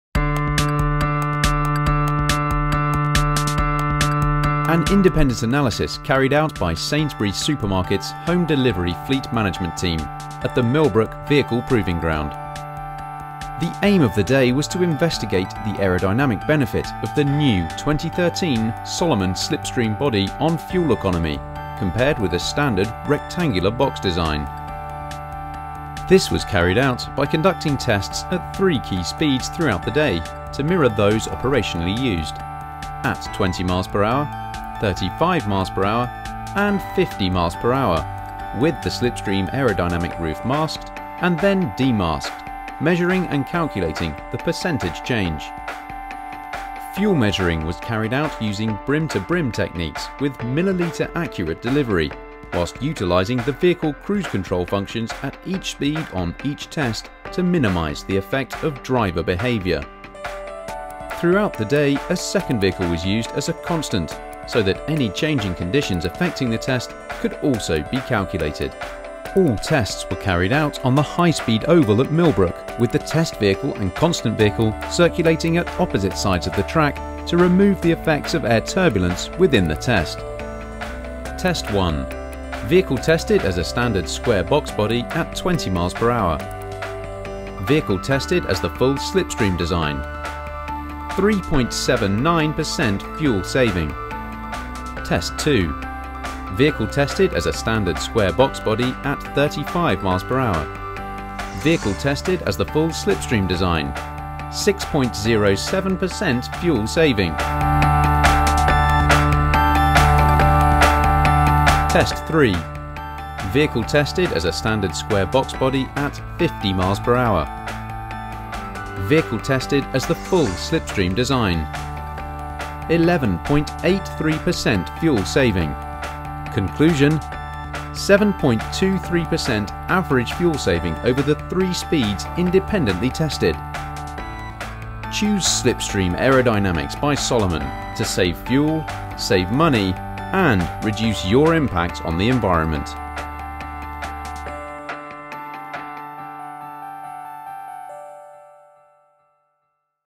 3.5T Test at Milbrook - Sainsbury's Independent Testing
Independent testing carried out by Sainsbury's supermarkets home delivery fleet management team at the Milbrook vehicle proofing ground.

The aerodynamic benefits of the Solomon's Slipstream body on fuel economy - 7.23% average fuel saving.

Save fuel, save money and reduce your impact on the environment.